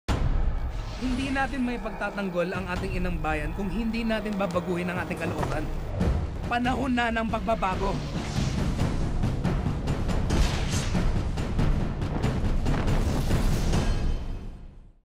"Panahon na ng pagbabago" -Andres
Abangan si Sid Lucero bilang Andres Bonifacio.